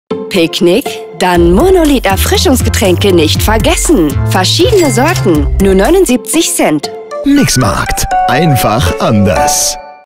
Kohlensäurehaltige Erfrischungsgetränke MONOLITH // Top Angebote Mix Markt 03.08.-08.08.2020
Picknick? MONOLITH-Erfrischungsgetränke nicht vergessen! Verschiedene Sorten - 79 Cent.